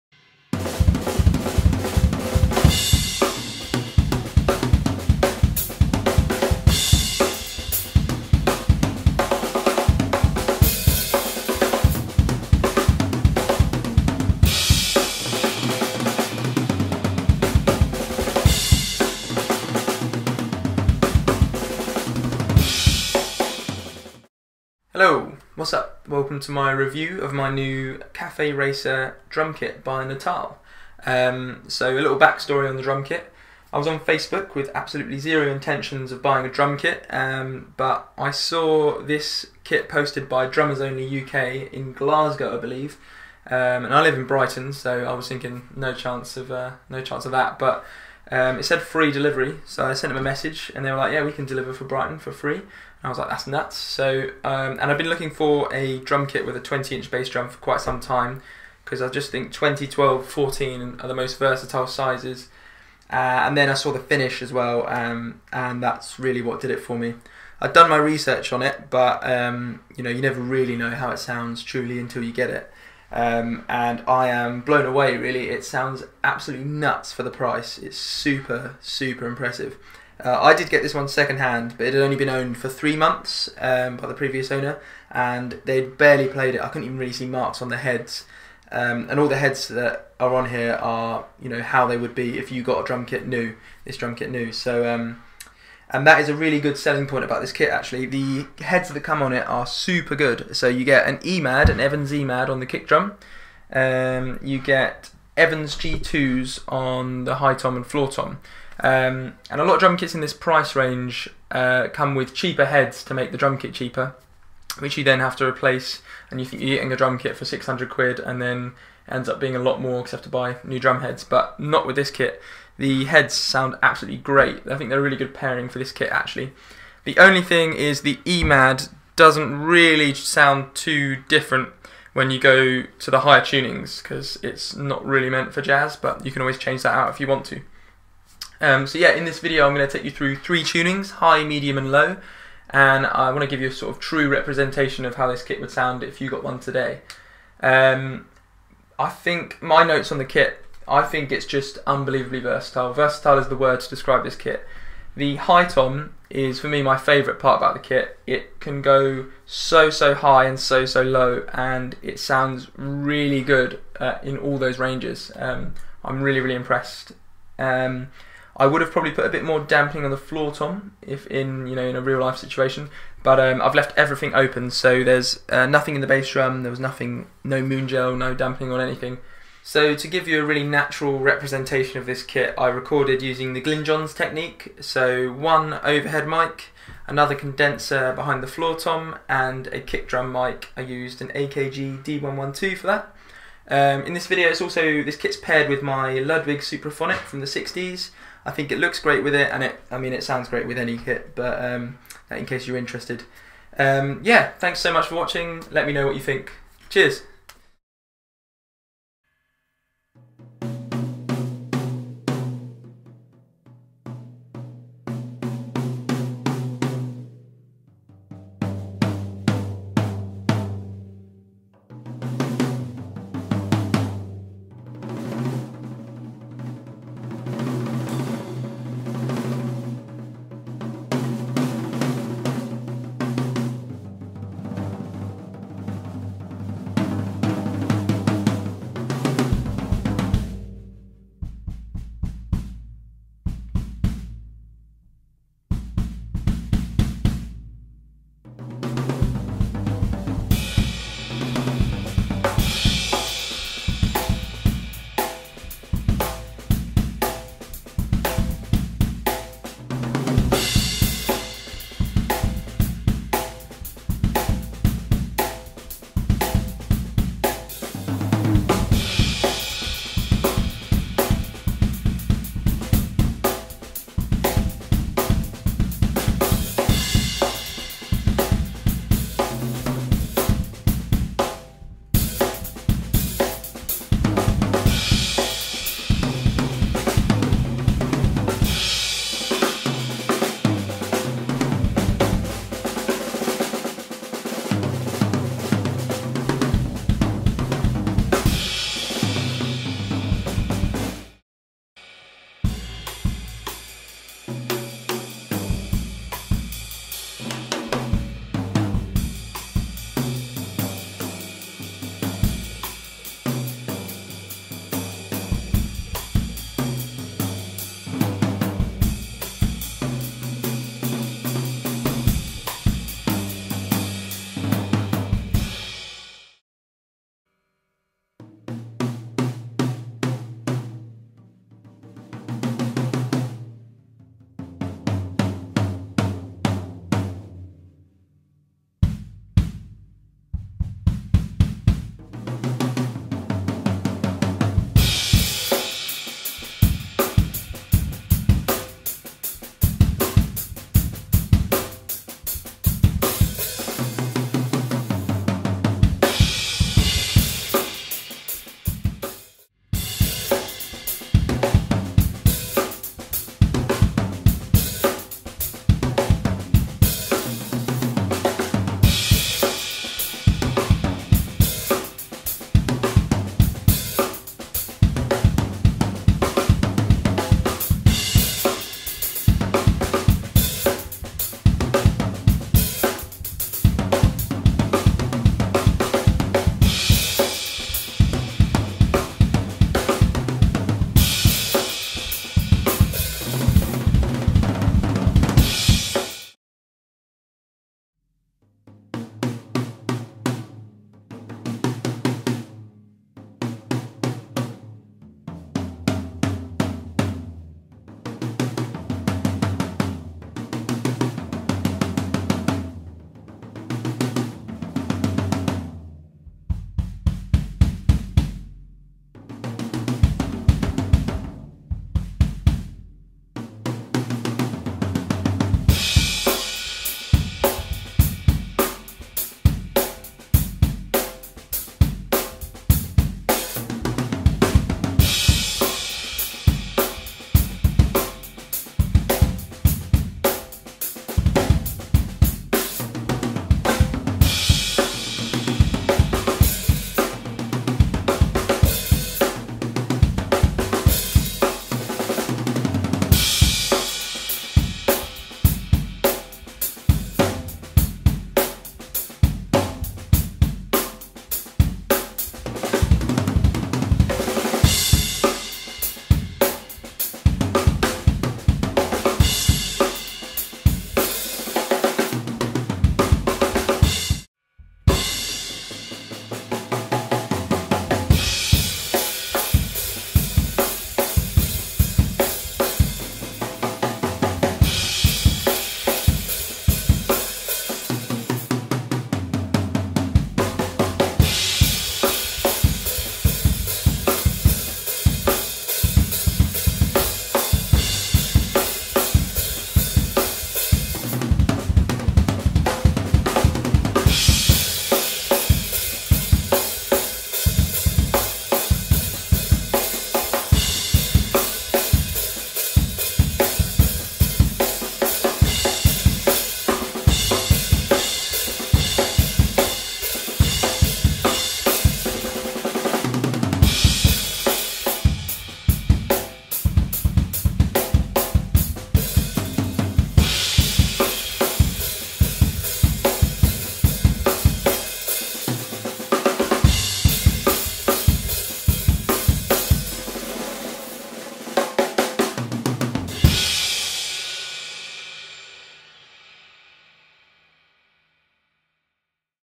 Natal Cafe Racer Review/Demo
Here's my review of my new kit from Natal.
I'm super impressed with this kit, this thing really is value for money. I kinda like the finish a bit too much too... 😂